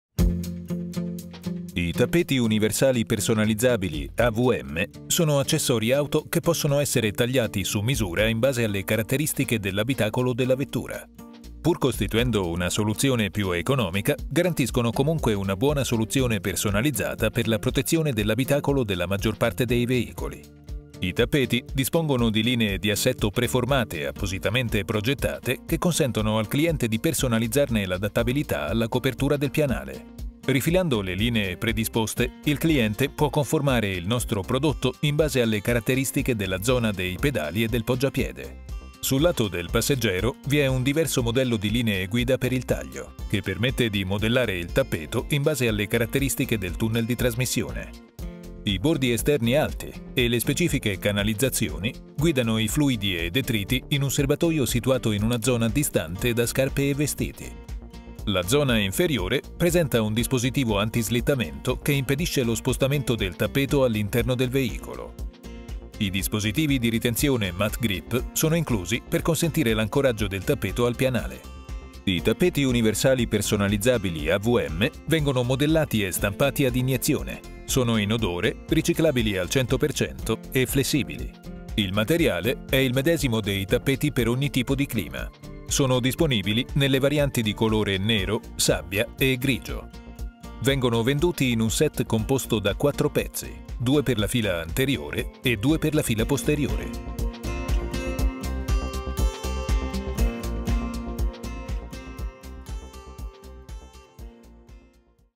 (Italiano) WeatherTech Tappeti Universal AVM
Per chi ha intenzione di investire per la protezione della propria auto un budget limitato, abbiamo voluto offrire qualcosa di veramente buono - il WeatherTech® AVM®.

Il WeatherTech® Tappeti Universal AVM® è l'evoluzione del tappetino "Universale" All-Weather. Questo prodotto è il più tecnologicamente avanzato tappeto per auto universale adattabile. I nostri progettisti si sono basati sull'analisi di più veicoli per determinare le migliori combinazioni di misure ideali, creando così le linee direttrici che  permettano la personalizzazione del nostro prodotto ad ogni veicolo.